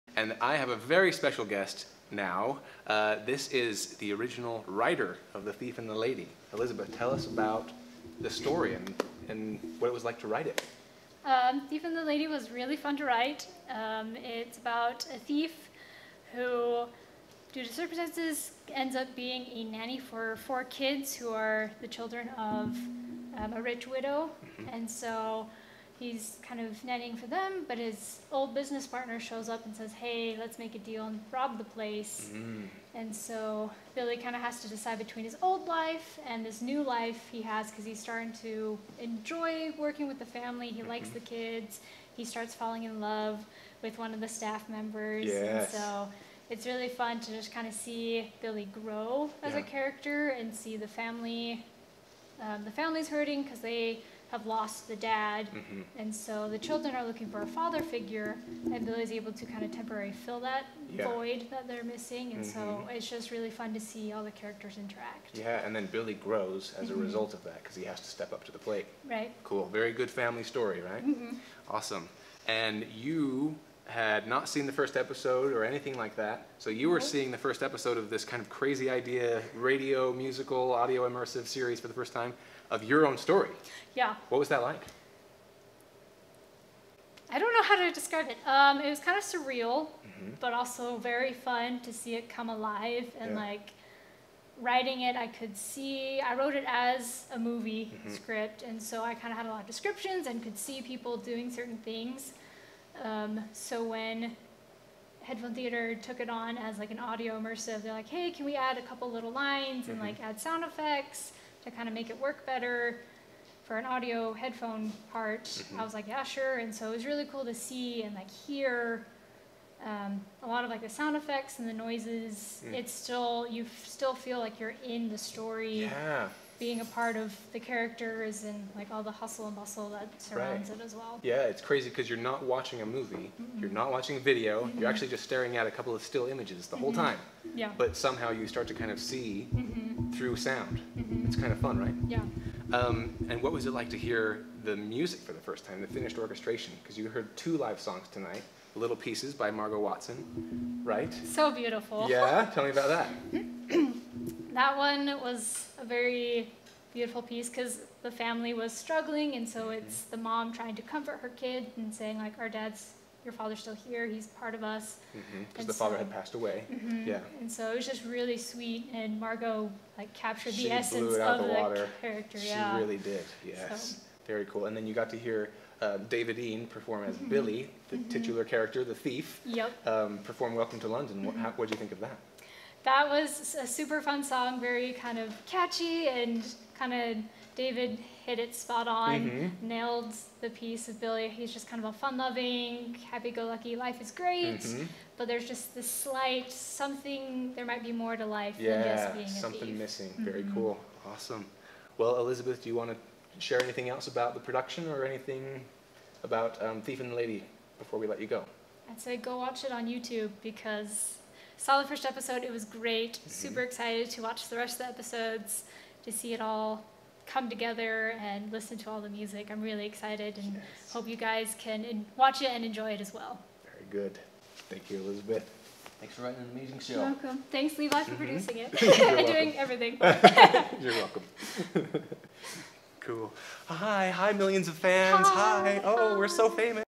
Thief Premier Interview - Elizabeth Adams
Elizabeth Adams wrote the original screenplay for The Thief and the Lady, an original musical series from Headphone Theater, with music and lyrics by Levi Taylor. Learn more about this and other original audio series